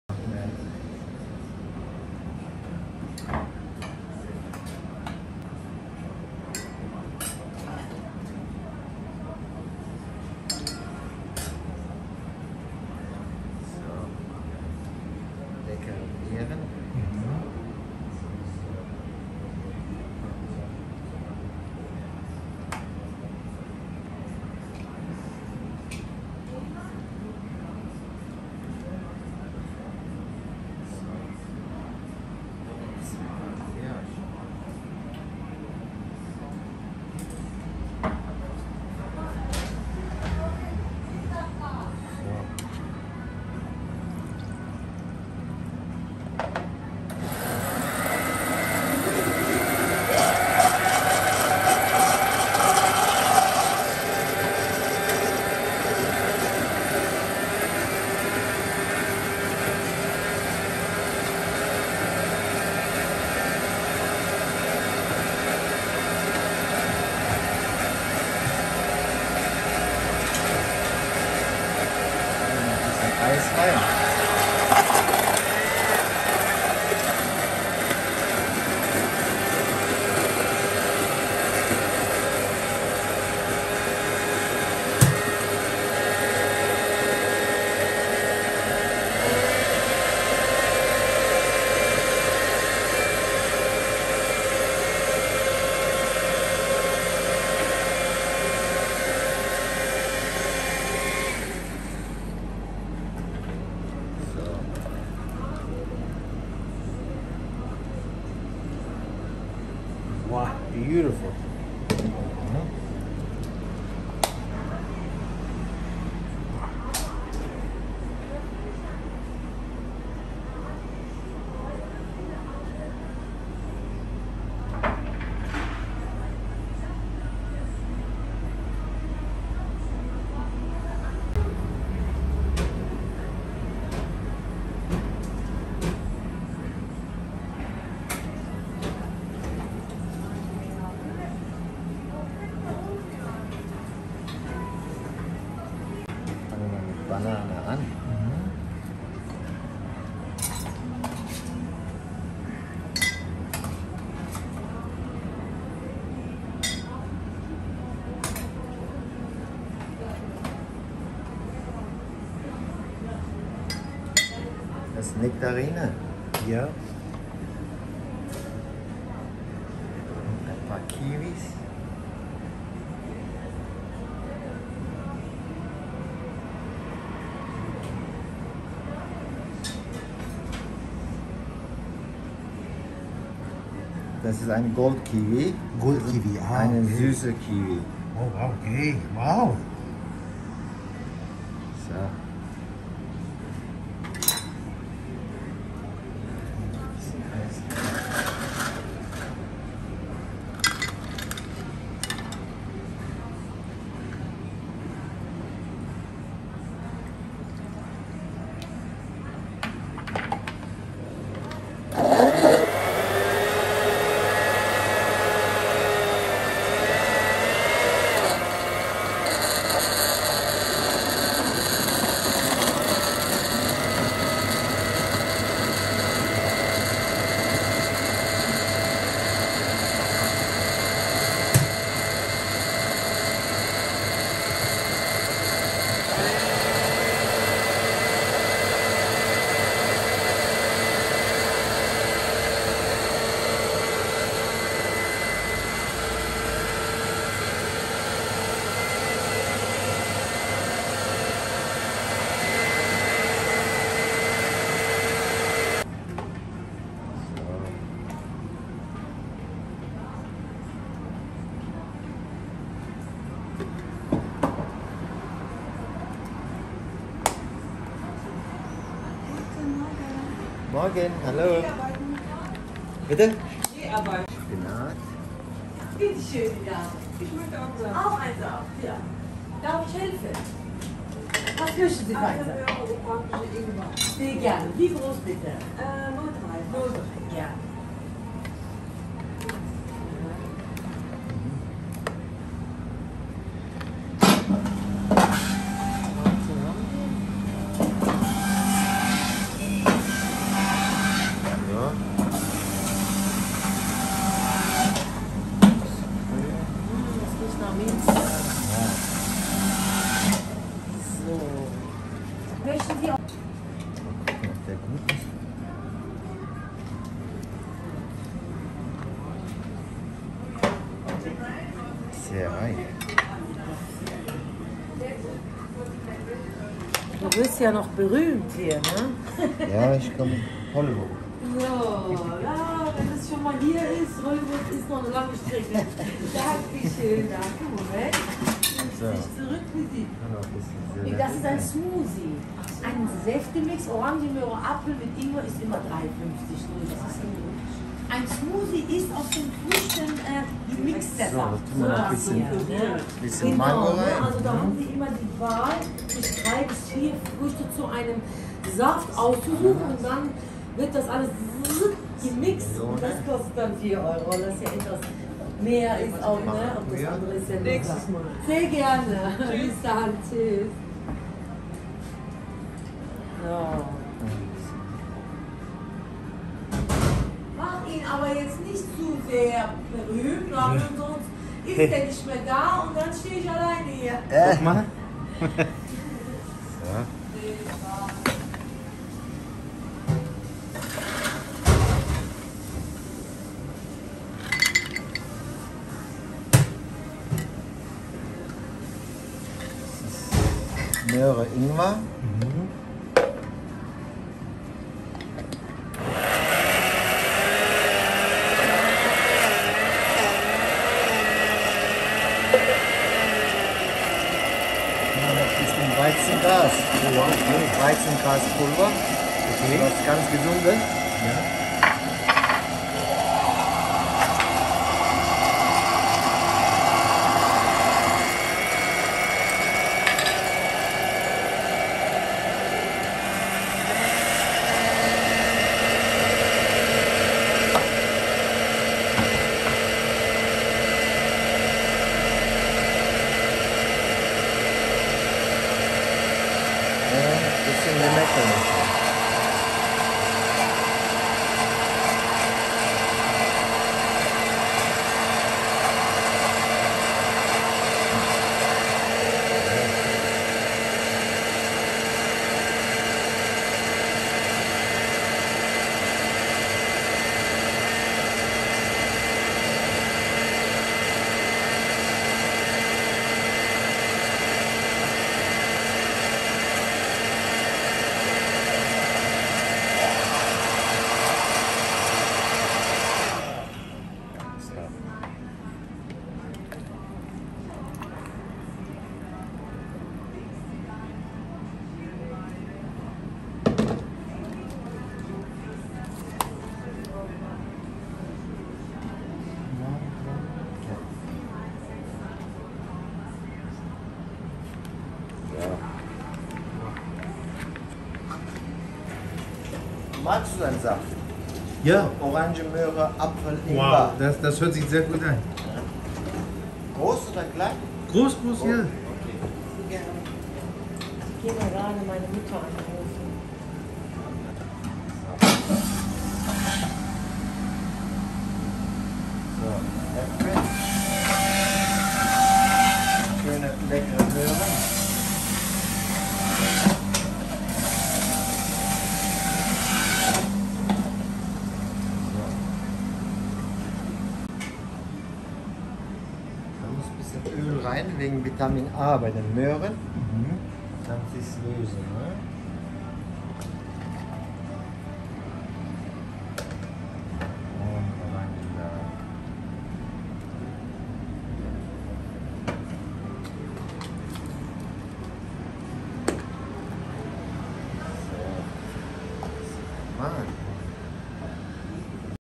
🔥 Fruit Juice Shop that makes the healthies juices! Juice Master in GERMANY 🇩🇪
🔥 Fruit Juice Shop that makes the healthiest juices, juice Master in GERMANY, we all try to stay healthy but sometiems we work too many hours and have too much stress. This juice shop makes really healthy juices. They buy their fruits daily and make juices dot their customers. The owner buys fresh fruits every morning from the market. The owner and his wife love the idea of living a healthy life!
This juice place is very popular for its varieties of healthy organic juice. No sugar is added! Watching the whole process of making the juice is very interesting to watch and I am sure you've never witnessed anything like that before.